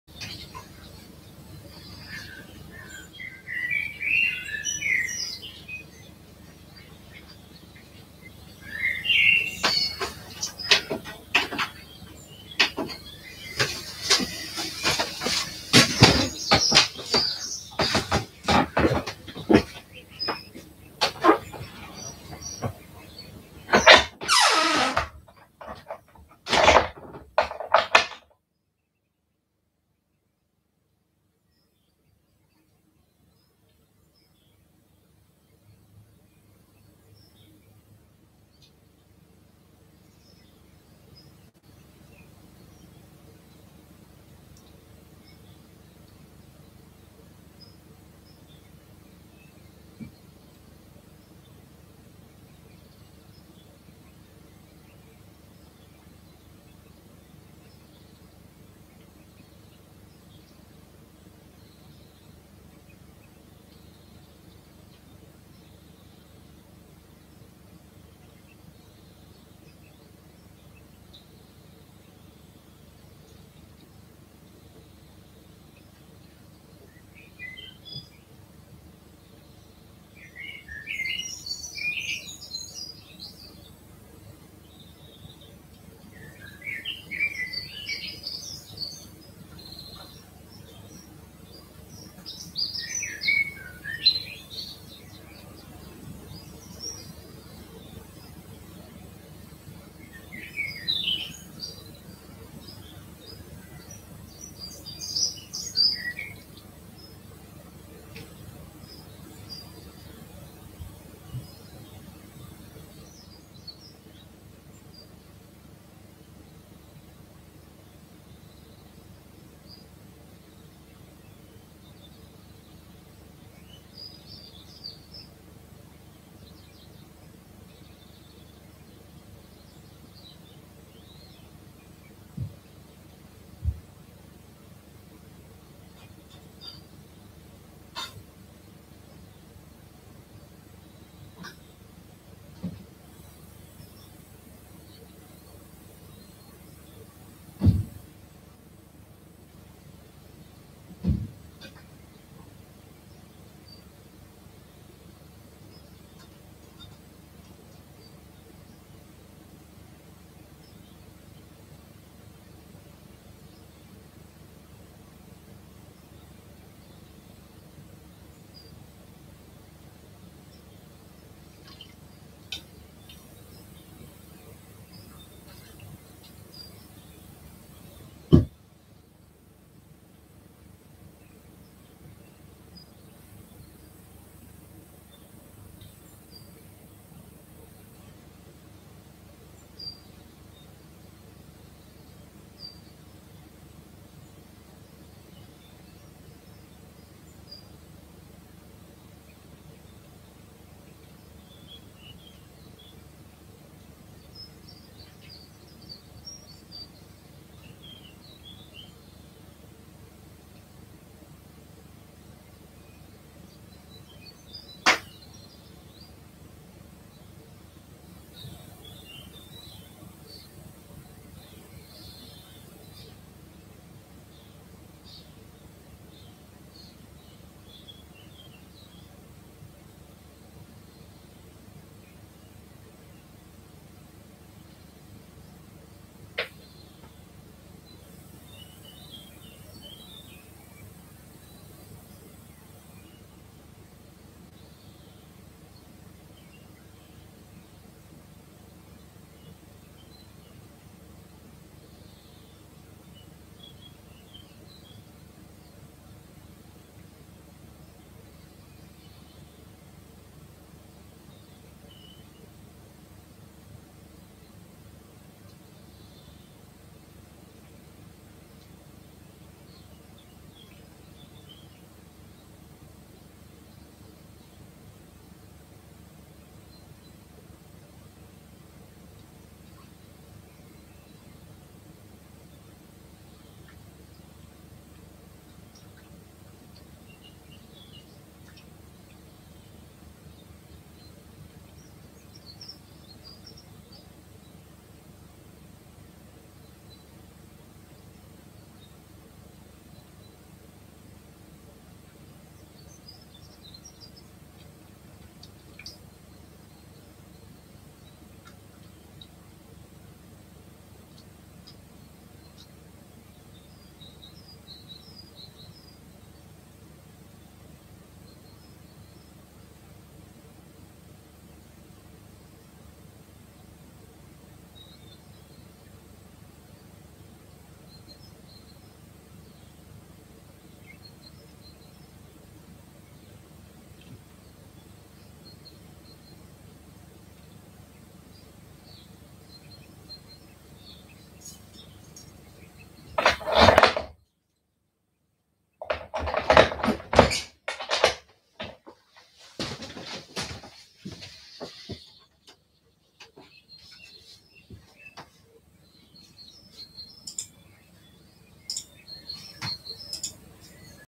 Duck TV Livestream
Four weeks old ducklings going about their day.